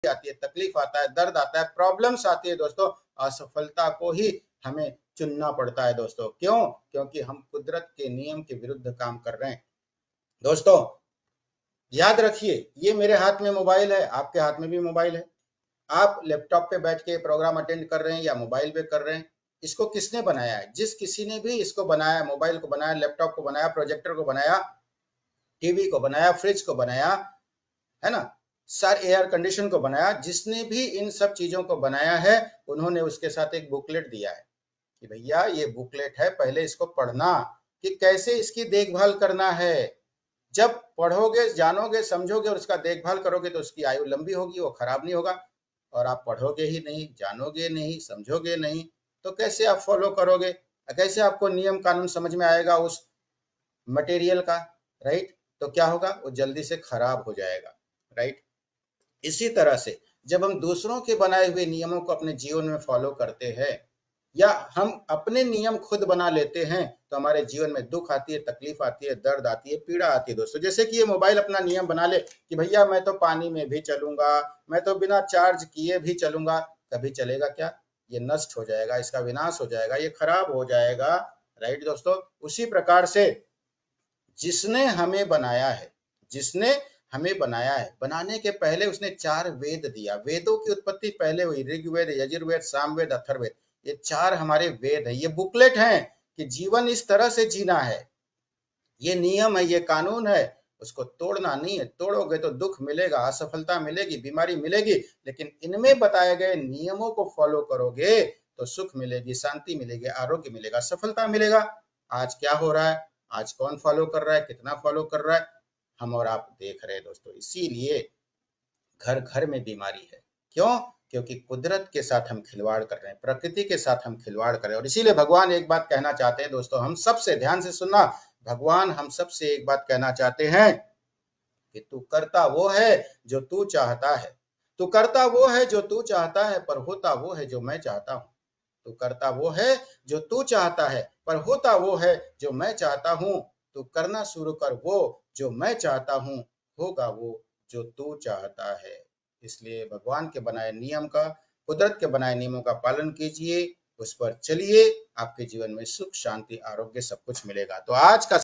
“कुदरत का नियम अपनाएं,सुख, शांति,स्वास्थ्य और सफलता पाएं"
मित्रो ,
नमस्कार।

“कुदरत का नियम" जान लें ,समझ लें तो जीवन बड़ा आसान हो जाता है। आज हम सब अपने नियम बनाकर जीते है या फिर किसी और के नियमो का पालन करते है इसीलिए जीवन में दुख ,तनाव,बीमारी और असफलता है। लेकिन जिसने हमें बनाया है, यदि हम उनके नियमो का पालन करते है, तो हमारे जीवन में सुख,शांति,आरोग्य और सफलता आयेगी।

इसीलिए भगवान इंसान से एक बात कहते है।

तू करता वो है, जो तू चाहता है,
पर होता वो है, जो मैं चाहता हूं।
तो फिर, तू करना शुरू कर वो ,
जो मैं चाहता हूं,
होगा वो, जो तू चाहता है।।

आपके समग्र स्वास्थ्य के लिए समर्पित,
डॉक्टर दयानंद तिवारी
हार्ट केयर,लाइफटाइल और माइंड बॉडी मेडिसिन एक्सपर्ट
वर्ल्ड क्लास हेल्थ और माइंड कोच,आयुर्वेदाचार्य,योगाचार्य, हीलर,ऑथर ,मोटिवेशनल स्पीकर